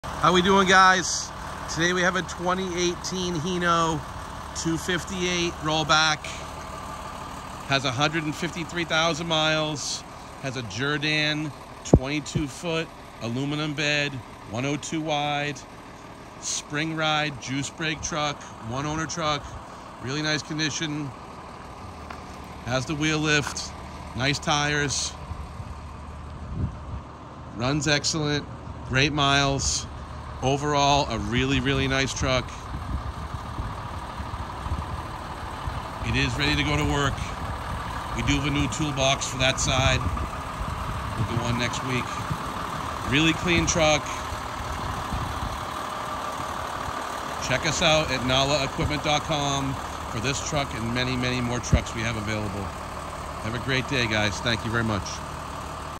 2018 Hino
2018 Hino 258 Rollback. Auto. Diesel. 153k miles. Jerr-Dan aluminum bed with wheel lift. 22’x102”.
Spring ride juice brake. 19.5 rubber. Stainless simulators. Two toolboxes. Very clean truck. Runs and drives excellent. Financing available to those that qualify. $69,950. Located in Upton MA. Thanks for looking.